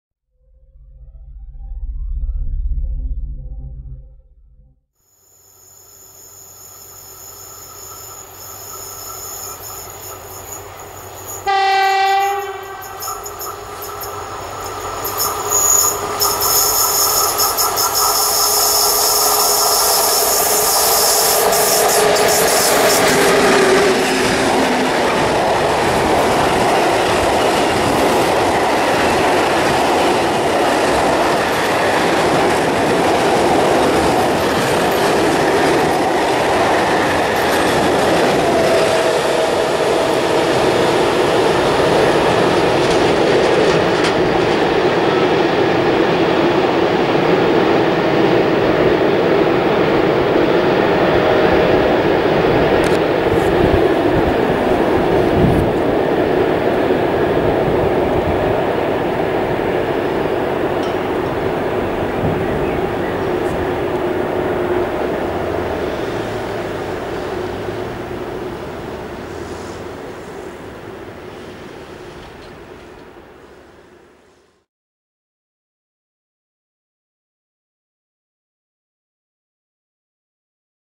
[CapTrain Polska] EU07-148 z BR232-01 | Zielona Góra
Oto przelot duetu EU07-148 z BR232-01 przez stację Przylep. Jest to również moje pierwsze nagranie EU07 CapTrain'u na odrzance :-) Pozdrawiam załogę :-) !
Zapraszam na mój fanpage na facebooku:
Następnie zachęcam do odwiedzin strony Kolejowa Zielona Góra na facebooku:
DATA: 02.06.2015